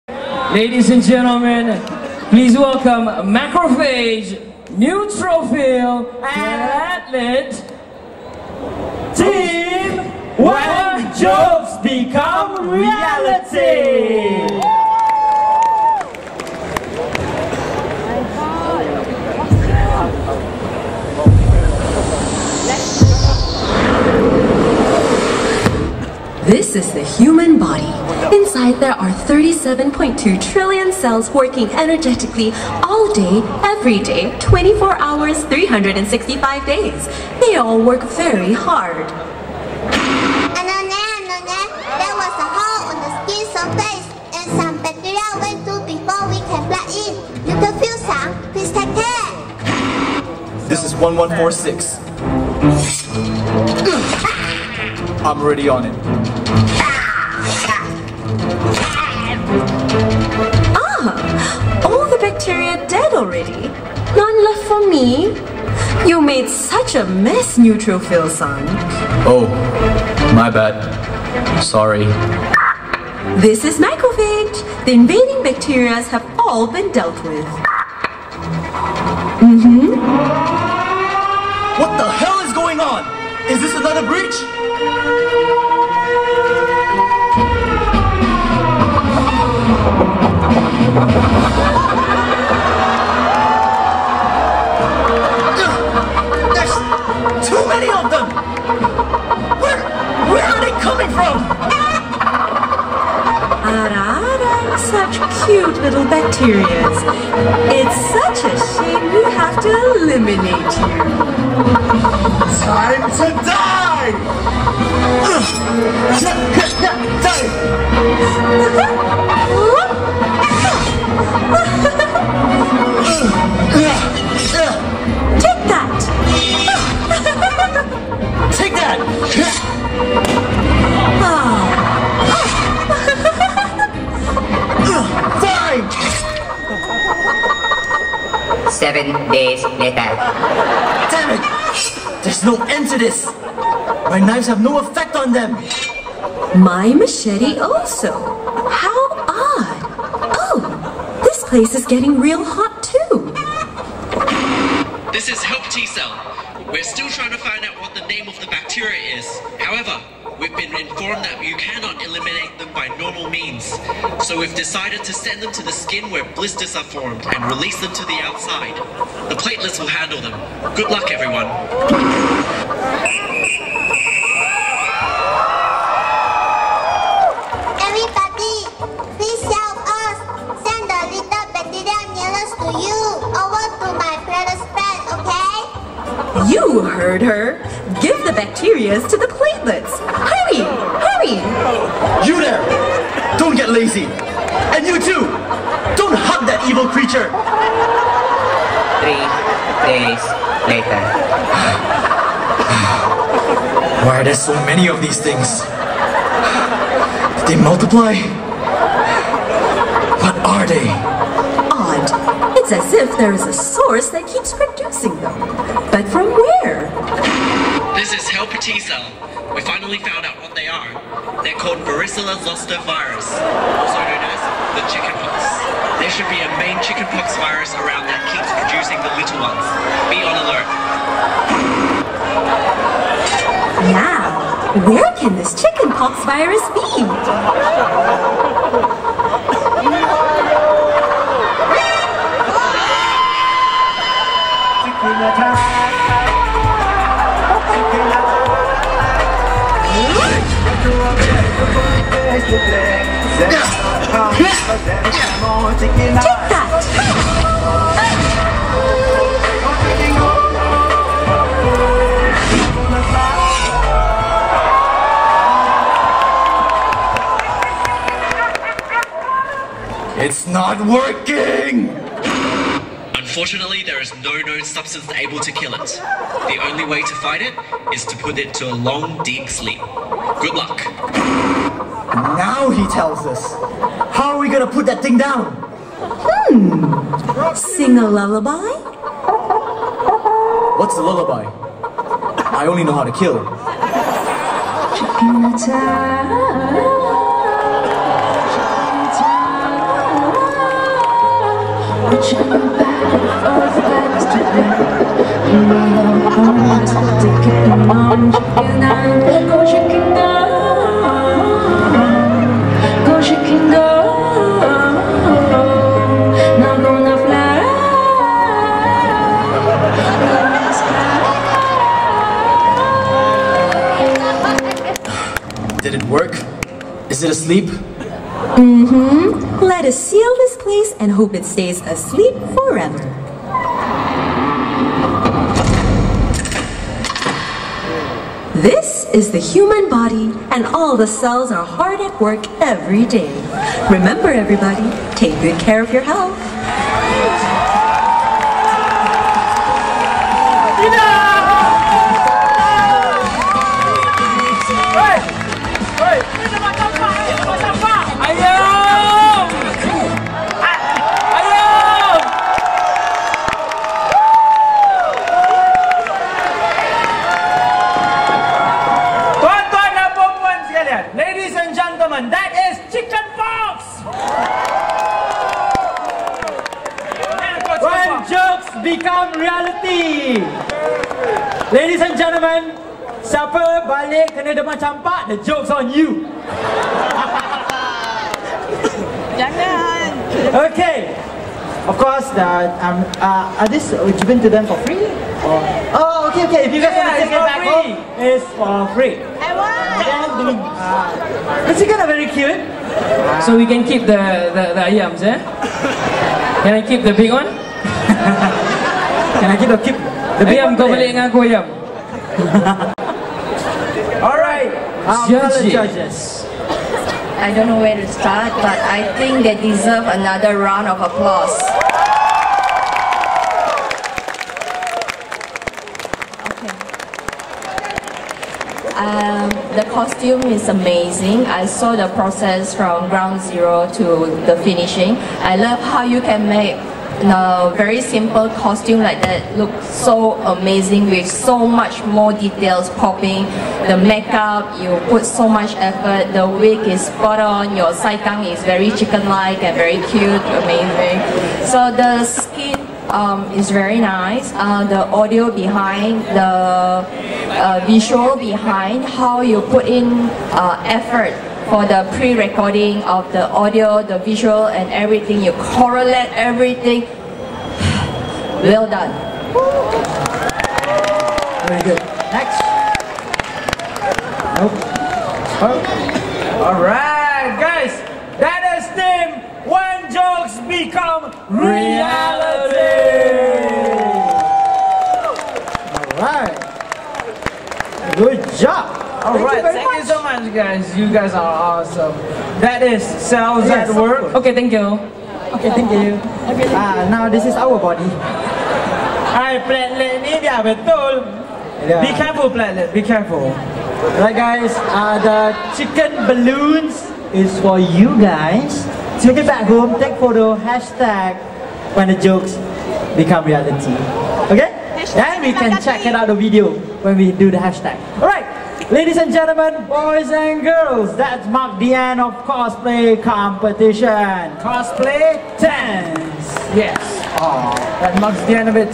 AniManGaki 2018 Cosplay Competition - Hataraku Saibo /Cells at Work [Champion] | HD
This team won Champion for the cosplay competition!

Event: AniManGaki 2018
Venue: Sunway Pyramid Convention Center
Date: 19 August 2018

Cosplayers:
White blood cell - Yuan
Macrophage - Sky Fara
Cell - KenjiMura
Platelet - Bibi Chan
Chicken pox - Ayam

Recorded using Panasonic video cam in 1920*1080 for 50 frames format.

Constructive comments are most welcome, hate comments will be deleted without notice.